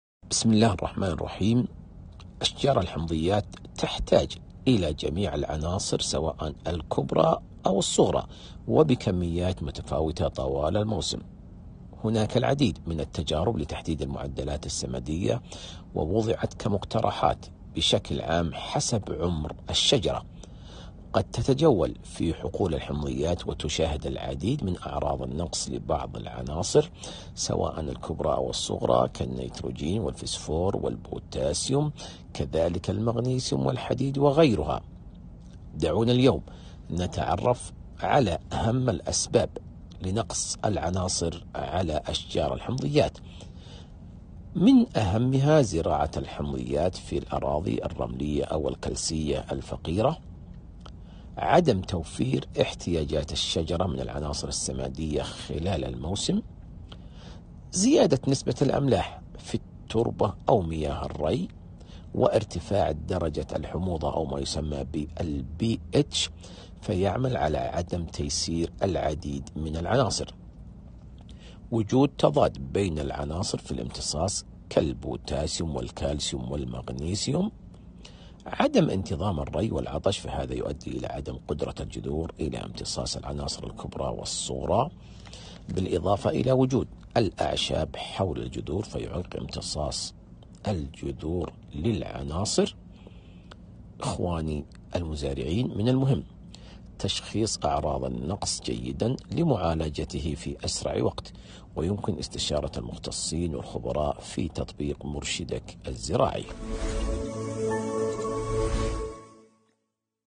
تعرف معنا على أهم الأسباب التي تؤدي لظهور نقص العناصر الكبرى والصغرى على أشجار الحمضيات يوسف القرزعي
مرشدك_الزراعي#
التنمية_المستدامة#
جربها_في_موسمها#